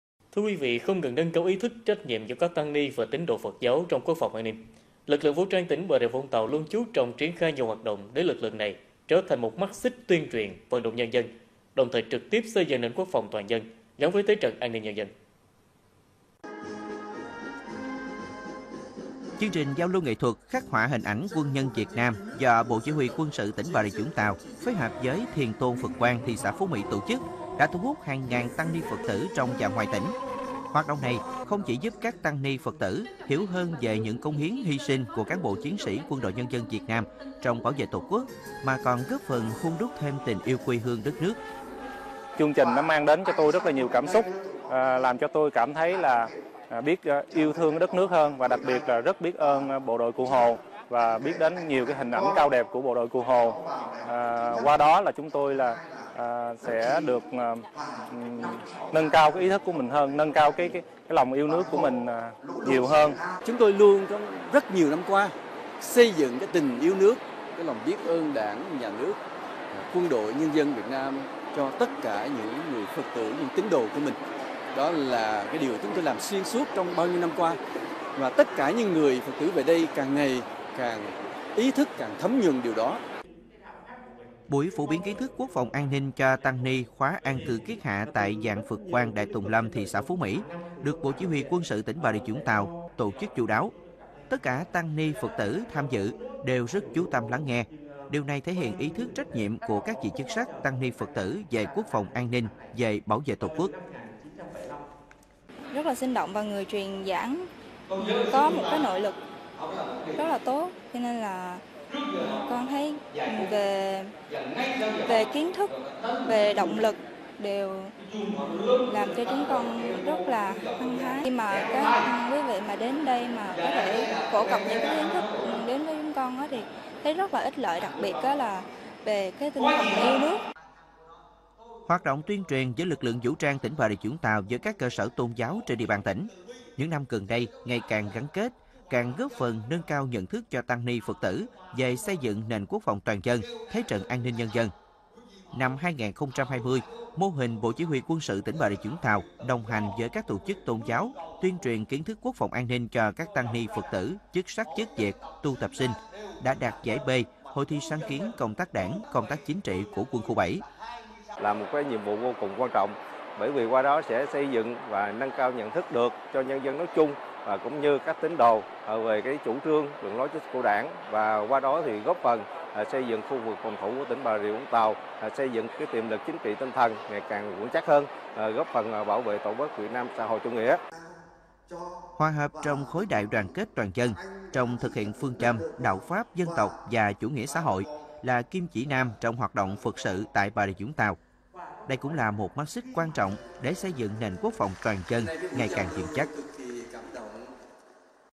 Thành Đạo 2021 || TĂNG NI, PHẬT TỬ BR-VT CHUNG TAY XÂY DỰNG NỀN QUỐC PHÒNG TOÀN DÂN
Nguồn: BRTgo